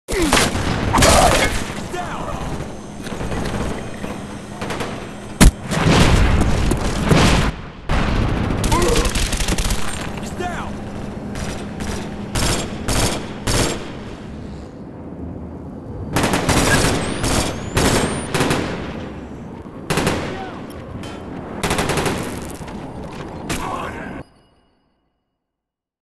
Fluke Tomahawk
Whilst on launch I attempted to tomahawk someone in last stand needless to say he was killed before the tomahawk hit him, things get weird from there.